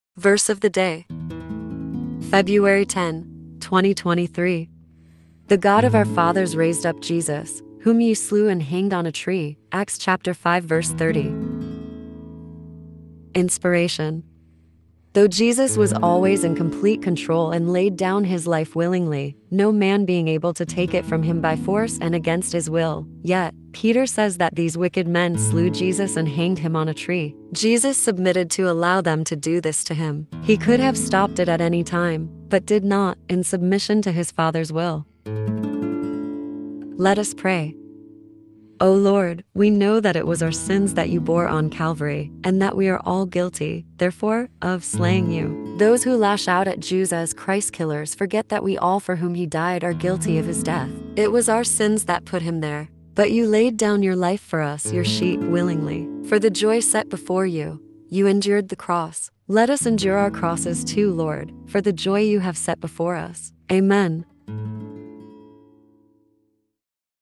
BIBLE VERSE OF THE DAY - February 10, 2023 | night prayer | Your Daily Dose of God's Word.
The God of our fathers raised up Jesus, whom ye slew and hanged on a tree.

Acts 5:30

INSPIRATION:
Though Jesus was always in complete control and laid down his life willingly, no man being able to take it from him by force and against his will, yet, Peter says that these wicked men "slew" (killed) Jesus and hanged him on a tree (the cross). Jesus submitted to allow them to do this to him. He could have stopped it at any time, but did not, in submission to his Father's will.

PRAYER:
O Lord, we know that it was our sins that you bore on Calvary and that we are all guilty, therefore, of "slaying" you. Those who lash out at Jews as "Christ killers" forget that we all for whom he died are guilty of his death. It was our sins that put him there. But you laid down your life for us, your sheep, willingly. For the joy set before you, you endured the cross. Let us endure our crosses too, Lord, for the joy you have set before us. Amen.

us